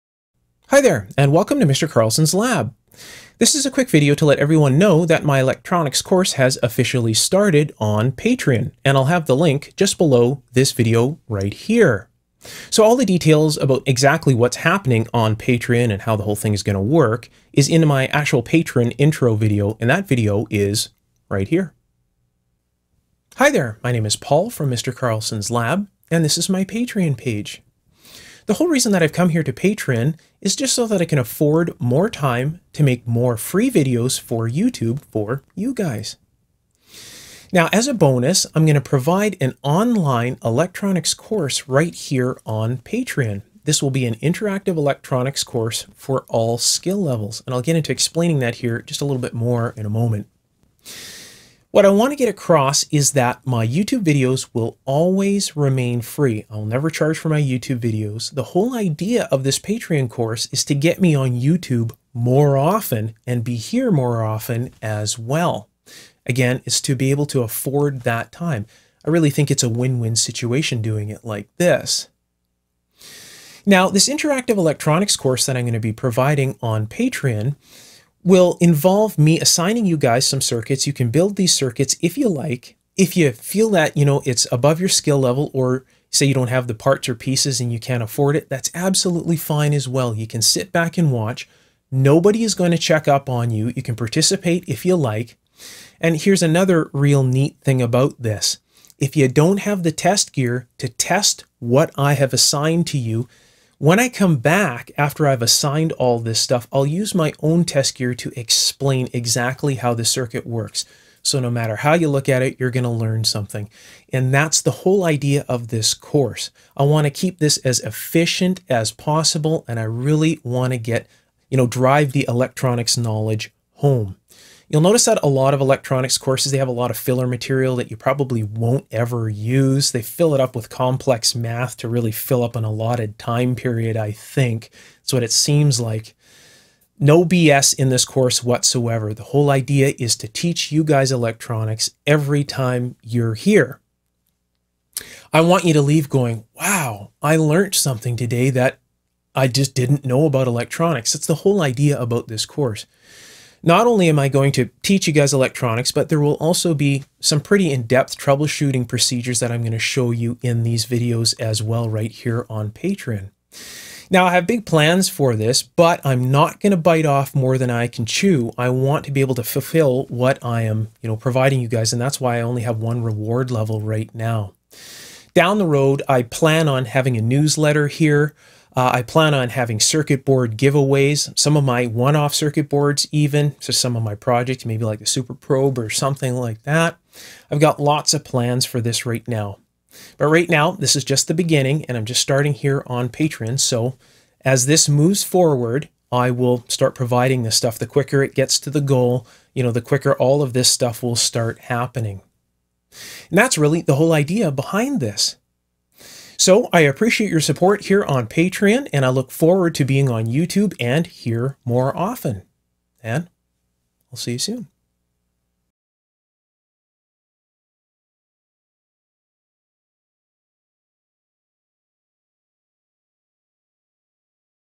Electronics Course Has Started, and Patreon Intro
Don't miss out! There will be lots of electronic devices and explanations you just won't find anywhere else. Modern electronic courses will not teach you most of this. Be a part of the group, and interaction.